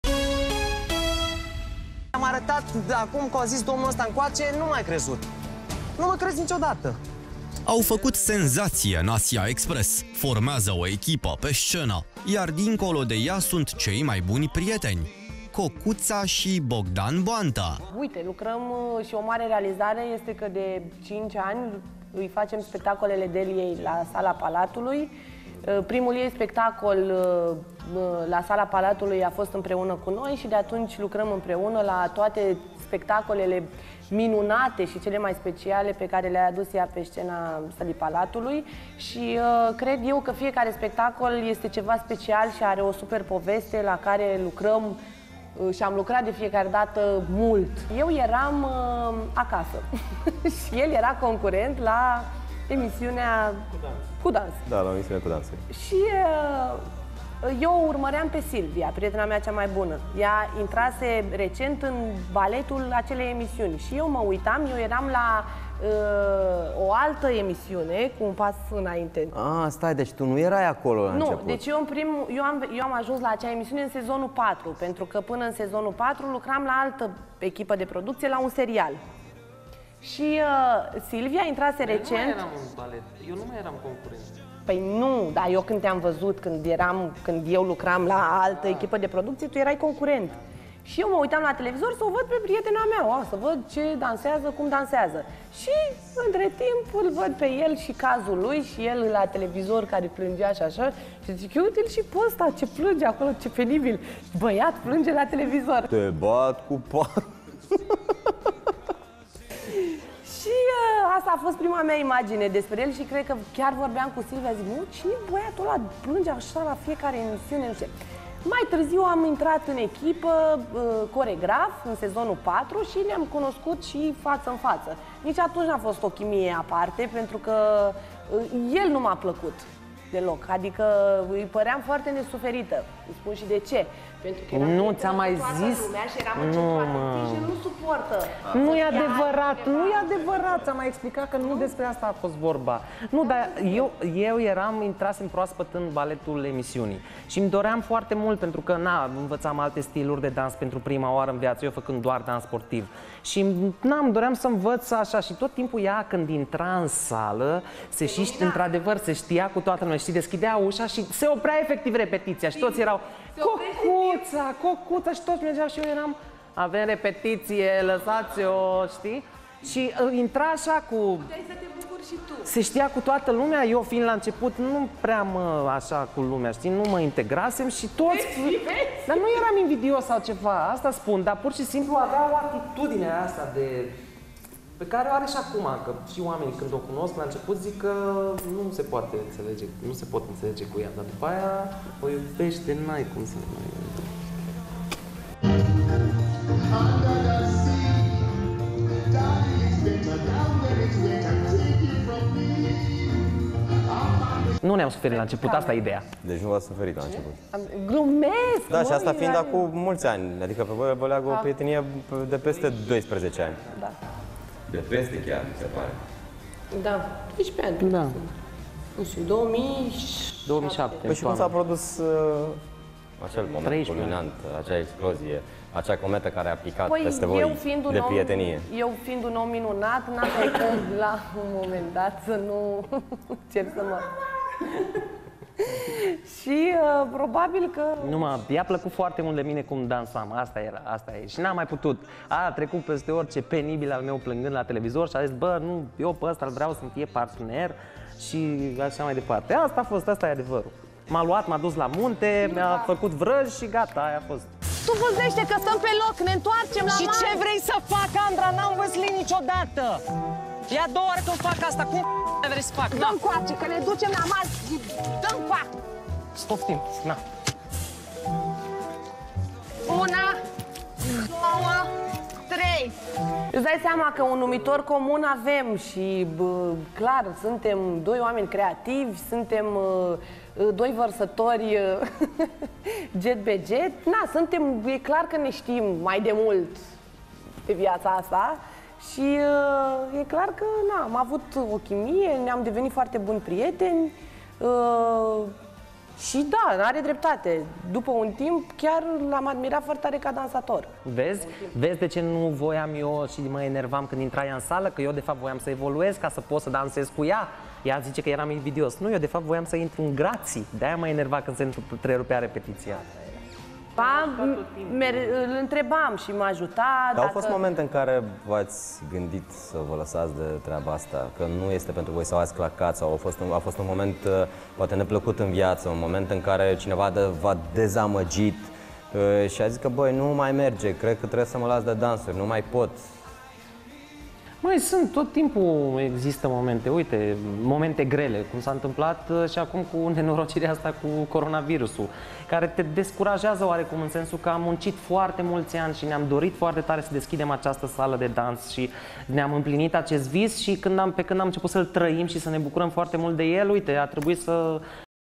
Cum au ajuns Cocuța și Bogdan Boantă cei mai tari coregrafi
Interviu în exclusivitate cu Cocuţa şi Bogdan Boantă. Ce dezvăluiri au făcut despre relaţia lor, dar şi cu ce probleme s-au confruntat în viață.